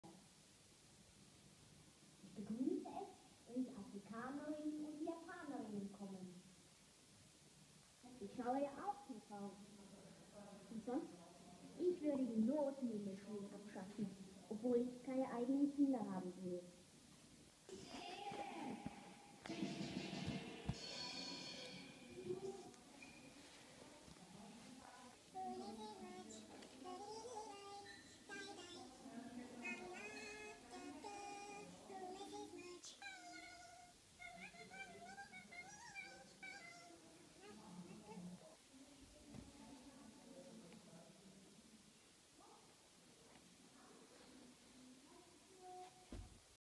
Art Installation: Das Zimmer (The Room) by Pipilotti Rist, 1962
Installation. A sofa, an armchair, a lamp, pictures, remote controls (all larger than actual size), and a TV monitor that shows a number of video works.